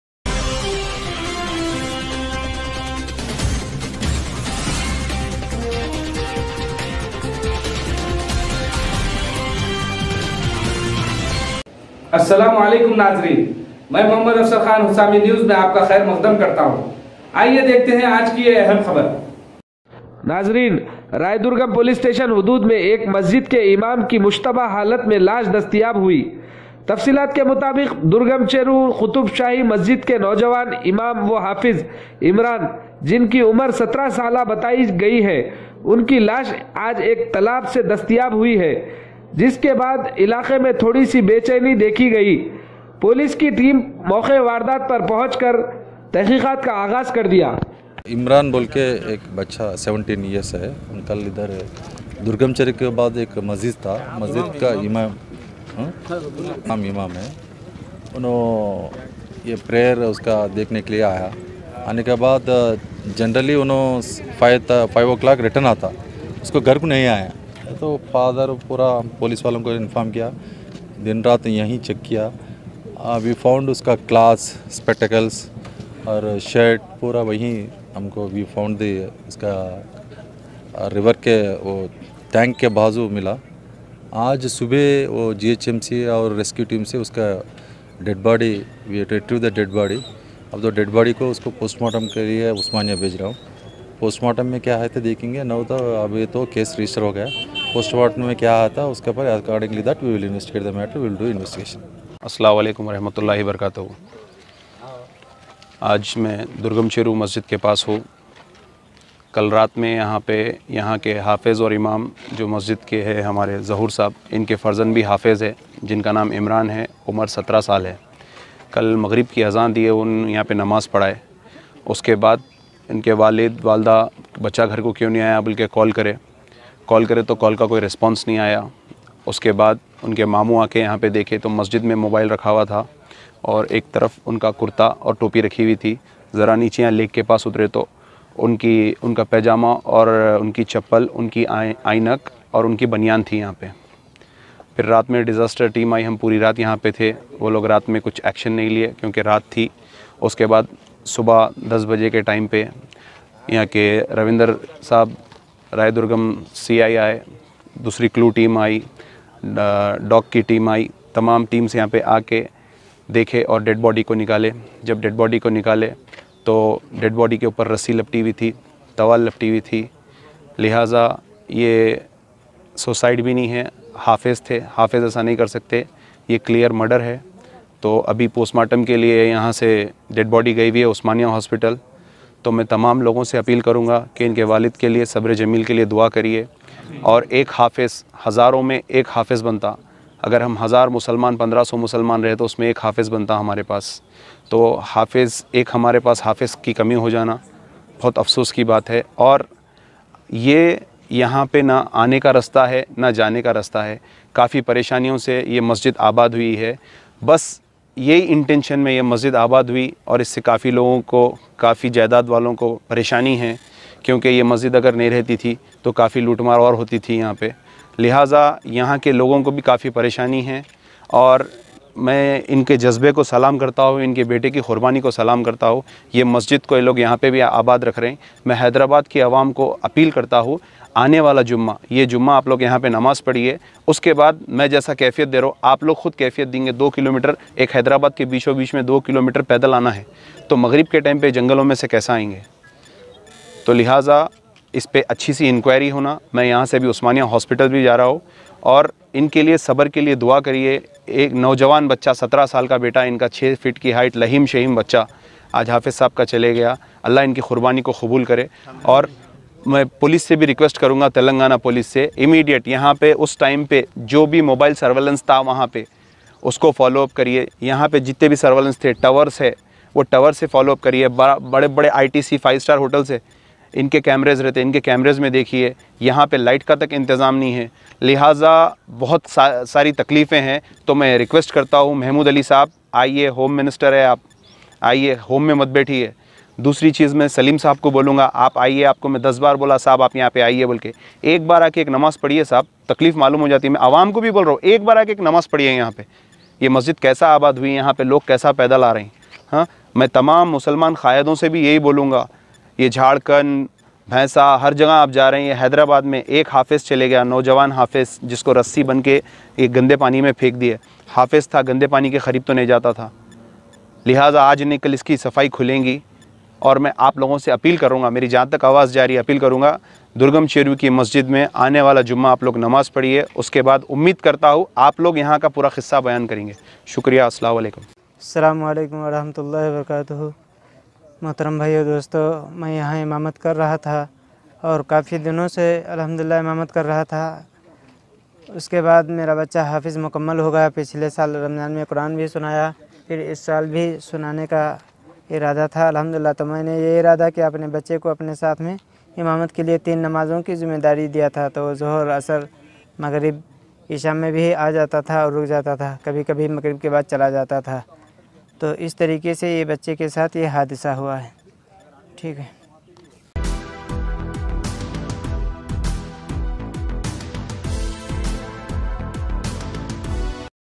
Qutub Shahi Masjid Imam Hafiz Imran Dead Body Found In Durgam Cheruvu Under Raidurgam PS Limits..
Husami News :Qutub Shahi Masjid Imam Hafiz Imran Dead Body Found In Durgam Cheruvu Under Raidurgam PS Limits..